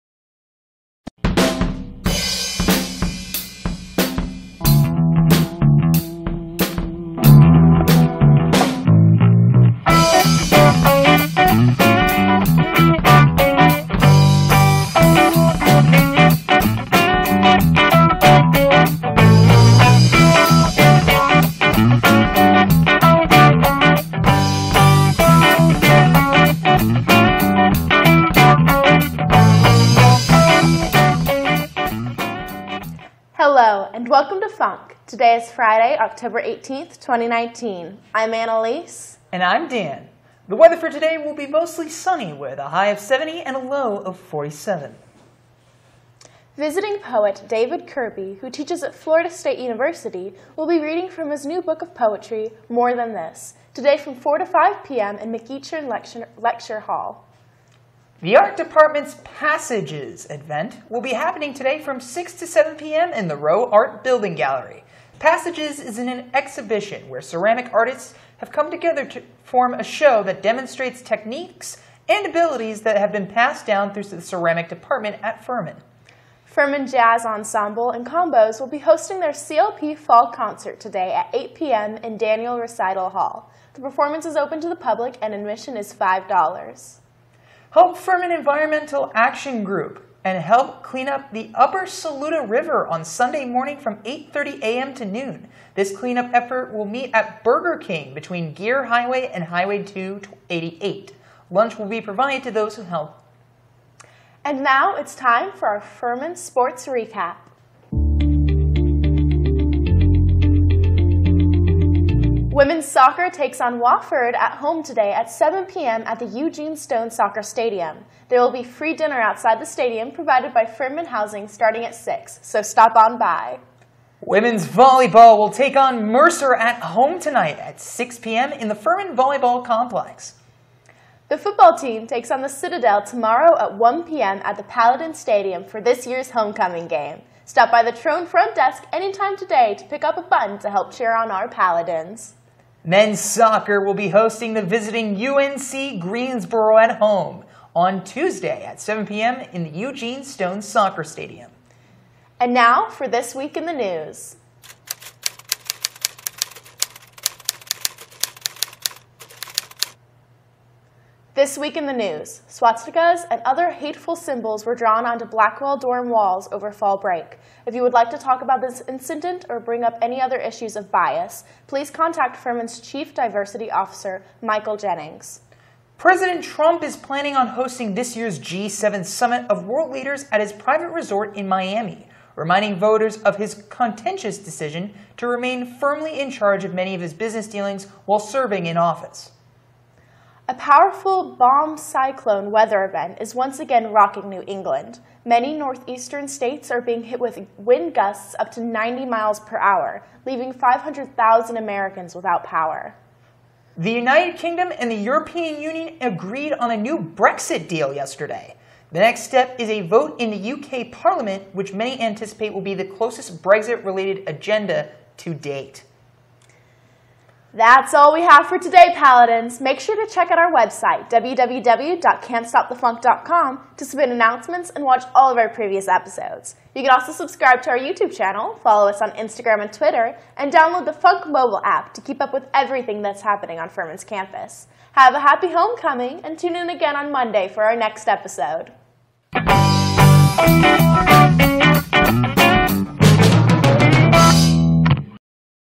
FUNC S3 E21 10/18/19
Happy Homecoming Paladins! To end off the week, Annalise and Dan give you the rundown for the weekend, alongside the sports recap and  "This Week In the News"